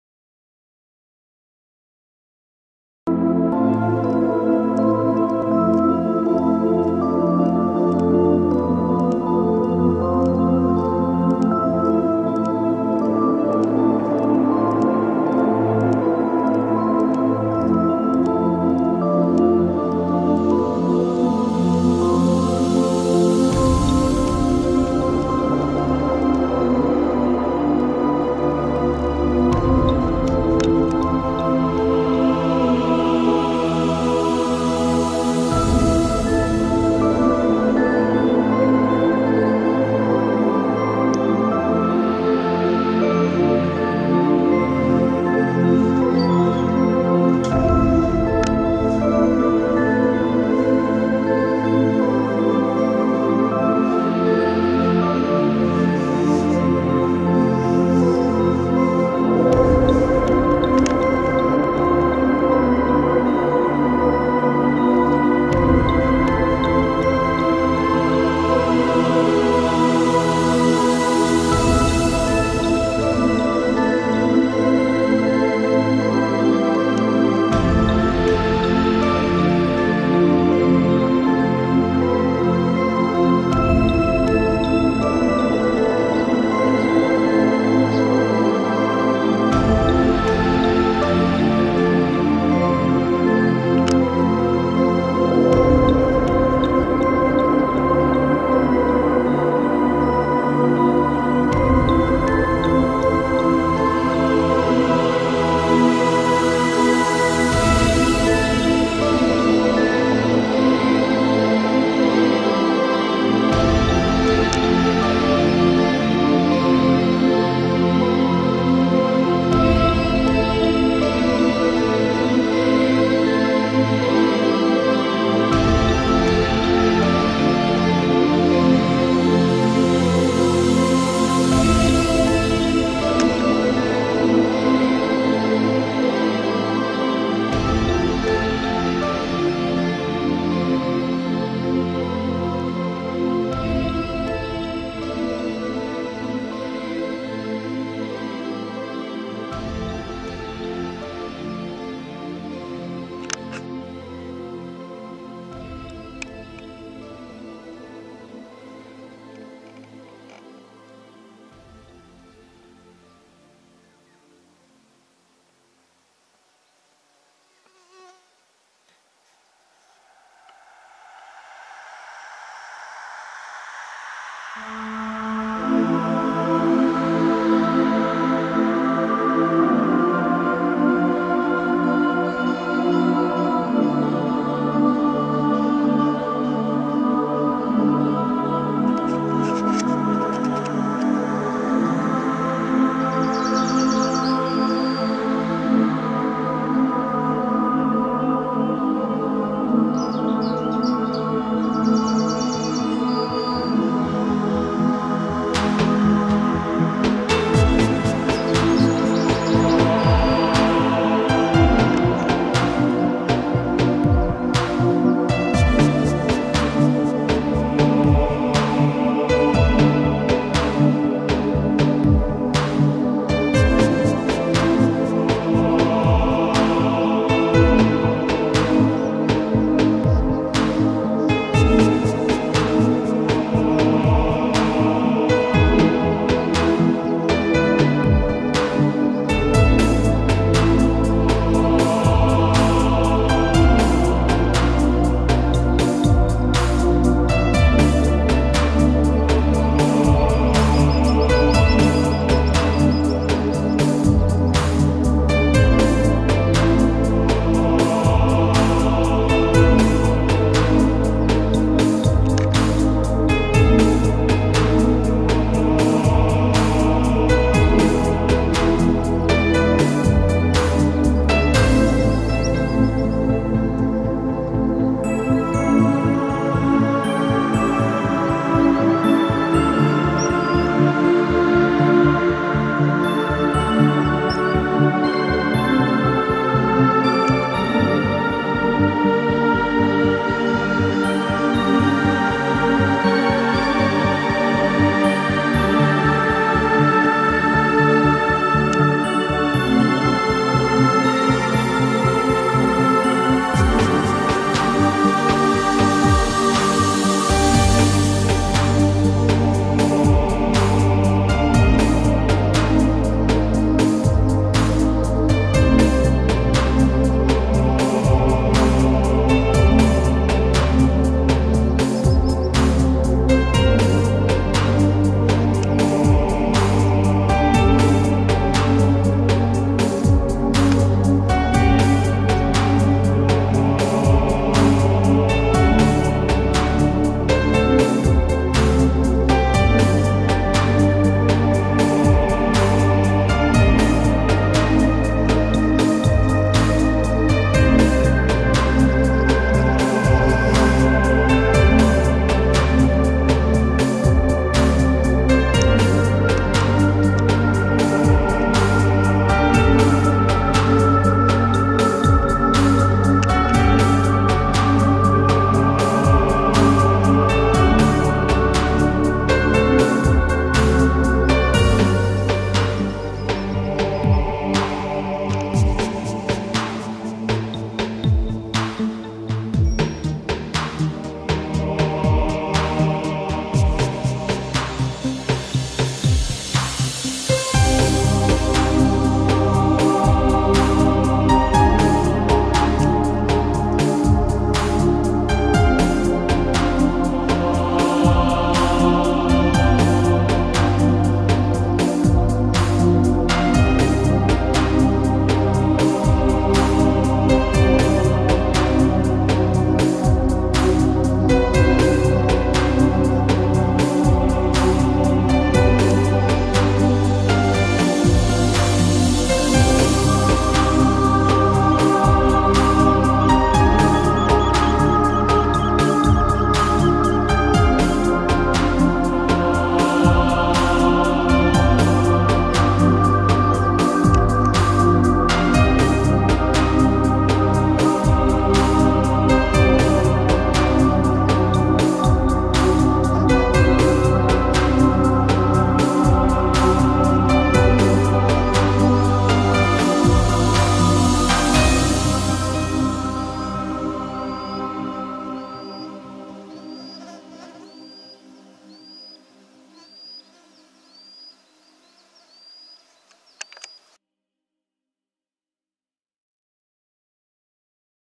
Уходящая весна на Алтае.
Алтай - май 2016 г Озеро Затон, река Чумыш, Алтайский край, Тальменский район.
Природа Алтая. Путешествия, поездки на автомобиле по красотам Горного Алтая.
The Nature Of Altai. Travel, car trips on the beauty of the Mountain Altai.